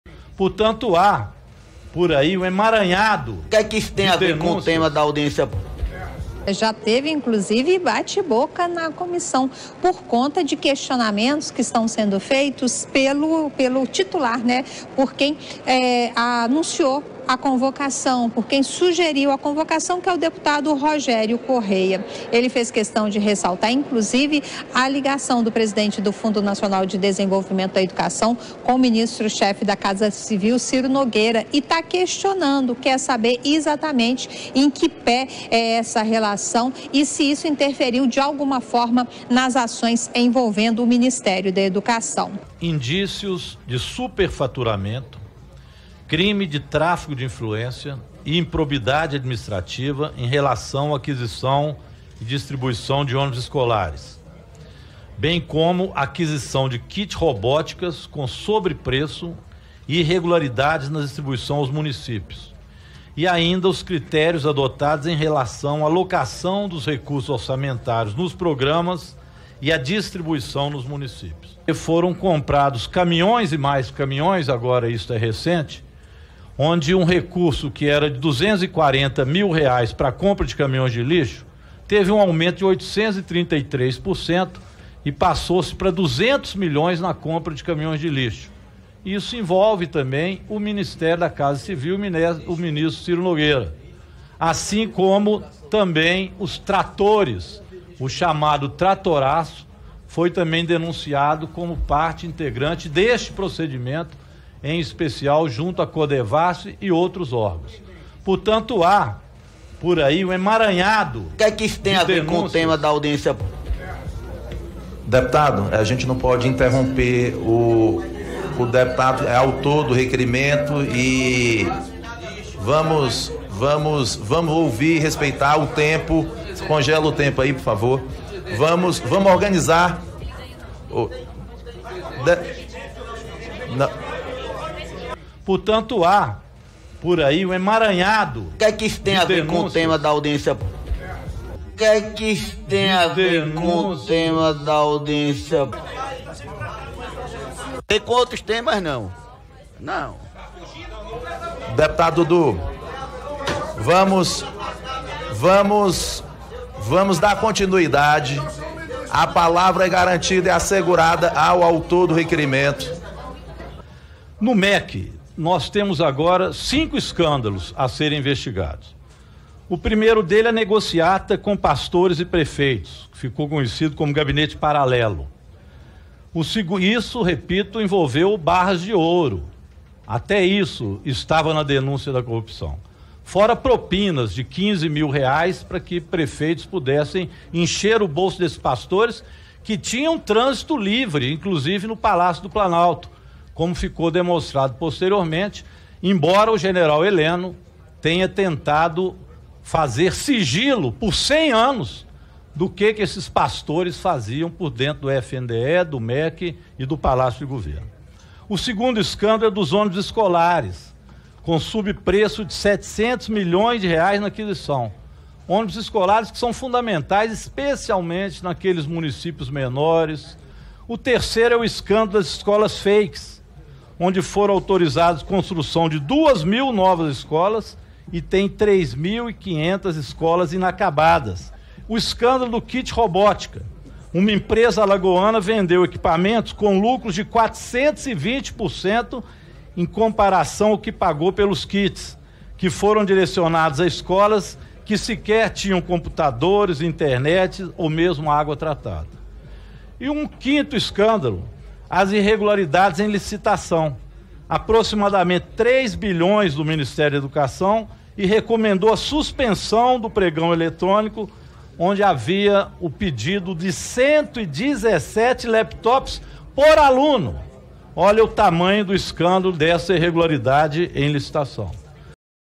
Deputado denunciava corrupção generalizada e o bolsonarista não aceitou...
CENTRÃO SENTIU...
O presidente do Fundo Nacional da Educação (FNDE) está sendo inquirido por deputados em Brasília, para responder (ou tentar) as inúmeras denúncias de corrupção envolvendo o órgão e o Ministério da Educação. Autor da convocação, o deputado Rogério Correia (PT-MG) passou a listar a série de delitos de corrupção generalizada no governo Bolsonaro. Um deputado do Centrão sentiu e deu piti... Confira.